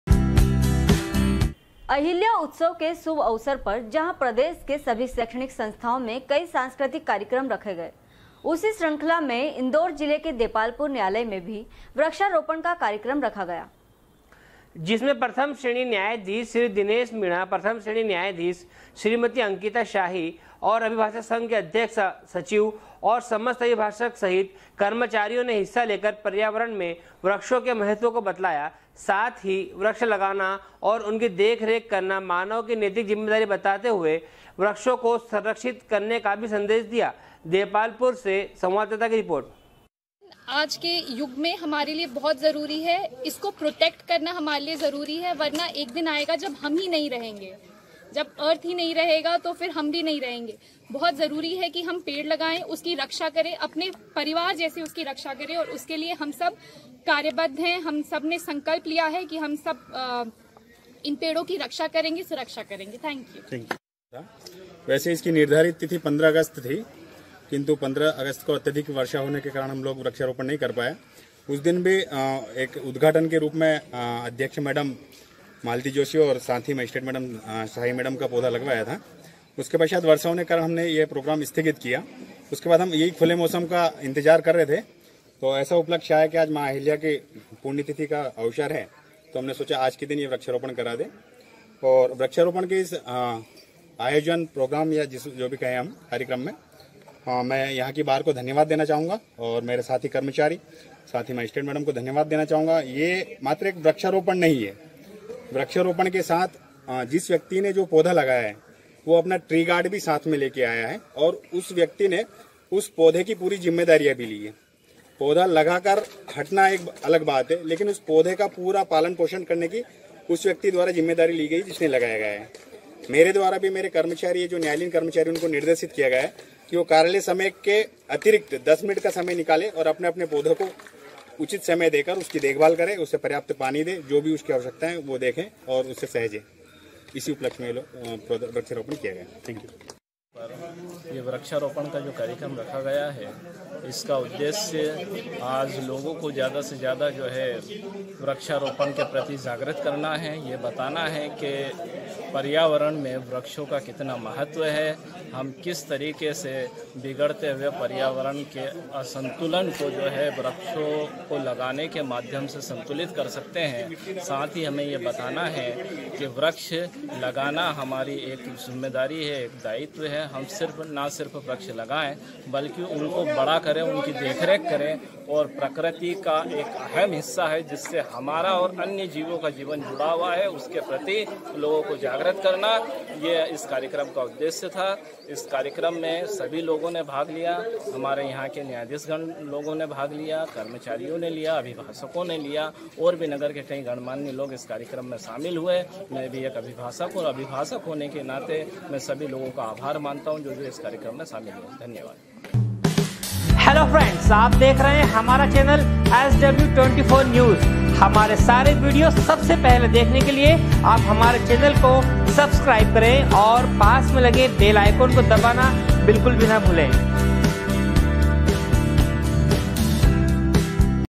sw24news depalpur, 30.08.2019
अहिल्या उत्सव के शुभ अवसर पर जहां प्रदेश के सभी शैक्षणिक संस्थानों में कई सांस्कृतिक कार्यक्रम रखे गए#sw24news